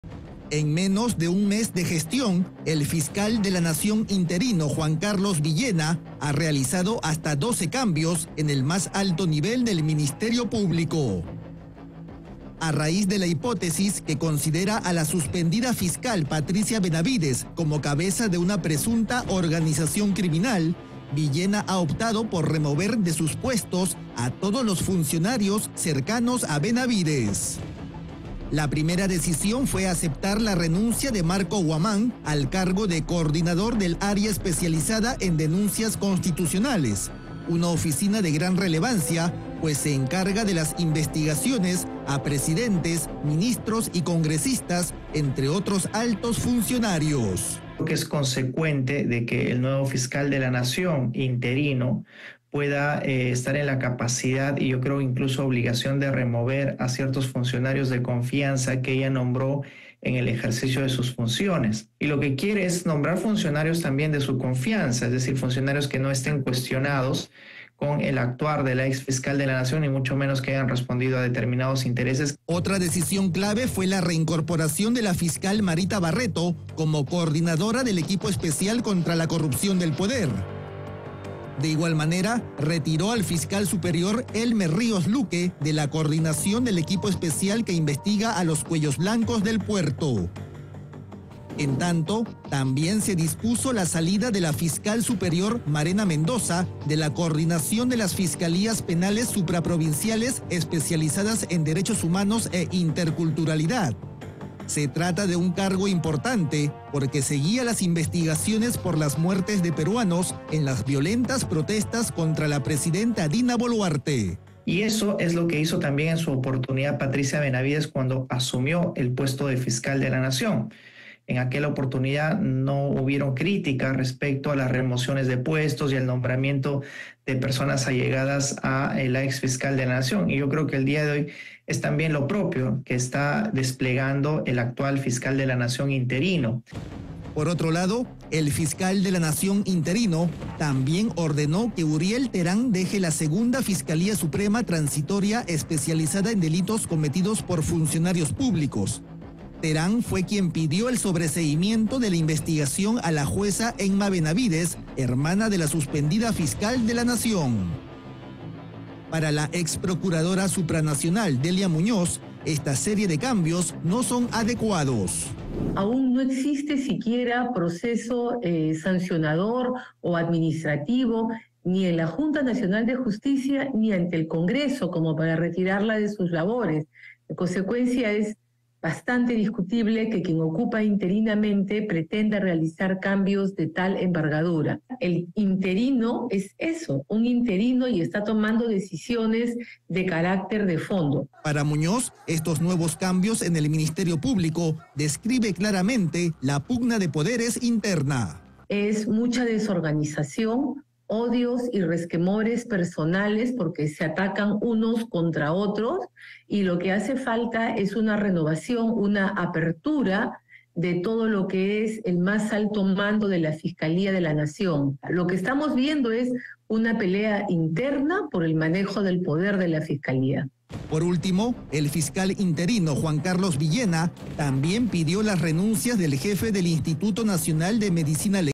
Fiscal de la Nación retira a 12 funcionarios cercanos a Patricia Benavides de sus cargos
En menos de un mes de gestión, el fiscal de la Nación interino, Juan Carlos Villena, ha realizado hasta 12 cambios en el más alto nivel del Ministerio Público, esto a raíz de la hipótesis que considera a la suspendida fiscal Patricia Benavides como cabeza de una presunta organización criminal, Villena ha optado por remover de sus puestos a todos los funcionarios cercanos a Benavides.

CAMBIOS EN EL MINISTERIO PÚBLICO

La primera decisión fue aceptar la renuncia de Marco Huamán al cargo de coordinador del área especializada en denuncias constitucionales, una oficina de gran relevancia, pues se encarga de las investigaciones a presidentes, ministros y congresistas, entre otros altos funcionarios.

Otra decisión clave fue la reincorporación de la fiscal Marita Barreto como coordinadora del Equipo Especial de Fiscales Contra la Corrupción del Poder (Eficcop). De igual manera, retiró al fiscal superior Elmer Ríos Luque de la coordinación del Equipo Especial que investiga a “Los cuellos blancos del puerto”.

También se dispuso la salida de la fiscal superior Marena Mendoza de la coordinación de las fiscalías penales supraprovinciales especializadas en derechos humanos e interculturalidad, se trata de un cargo importante porque seguía las investigaciones por las muertes de peruanos en las violentas protestas contra la presidenta Dina Boluarte.

Por otro lado, el fiscal de la Nación interino también ordenó que Uriel Terán deje la Segunda Fiscalía Suprema Transitoria especializada en delitos cometidos por funcionarios públicos. Terán fue quien pidió el sobreseimiento de la investigación a la jueza Emma Benavides, hermana de la suspendida fiscal de la Nación.

Por último, el fiscal interino, Juan Carlos Villena también pidió las renuncias del jefe del Instituto Nacional de Medicina Legal, y de los gerentes y asesores de la Fiscalía de la Nación.